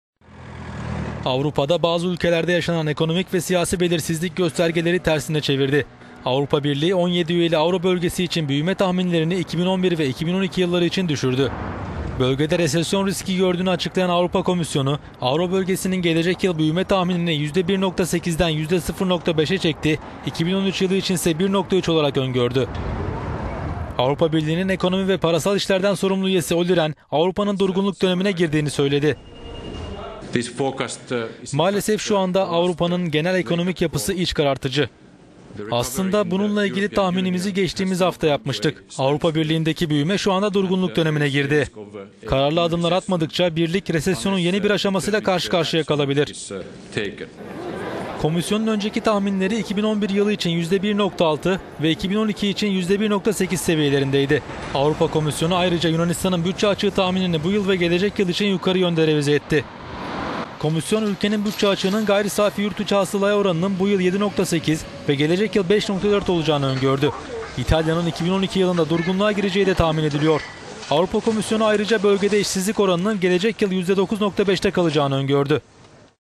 Avro Bölgesi büyüme tahminleri tersine döndü
Avrupa'da bazı ülkelerde yaşanan siyasi ve ekonomik belirsizlik, göstergeleri tersine çevirdi. Avrupa Birliği, 17 üyeli Euro Bölgesi için büyüme tahminlerini 2011 ve 2012 yılları için düşürdü.

Bölgede resesyon riski gördüğünü açıklayan Avrupa Komisyonu, Avro Bölgesi'nin gelecek yıl büyüme tahminini yüzde 1,8'den yüzde 0,5'e çekti, 2013 yılı için ise 1,3 olarak öngördü.